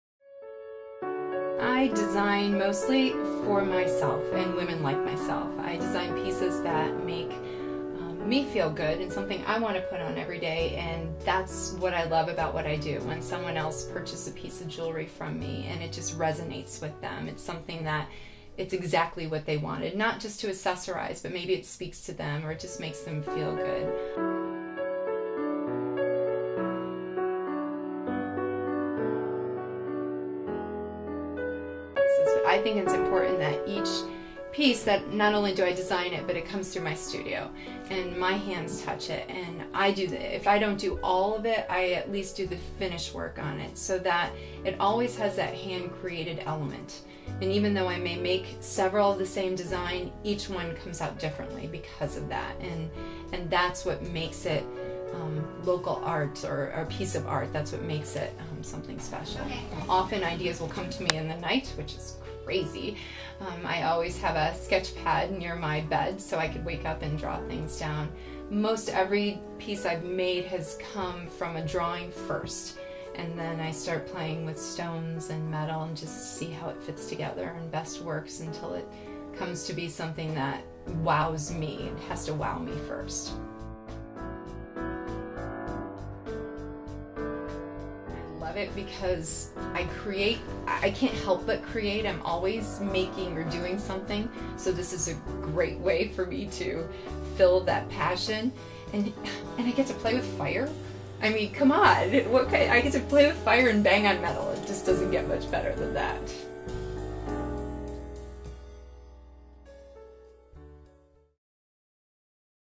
Lisa J Lehmann Artist Metal Smith Studio Jewel welcome to my Studio
An introduction to me Lisa j Lehmann the gold smith, jewelry artist behind Studio Jewel.

This is why I do what I do - what I am passionate about - and who I design for.

Come in, stay awhile, I would love to meet you!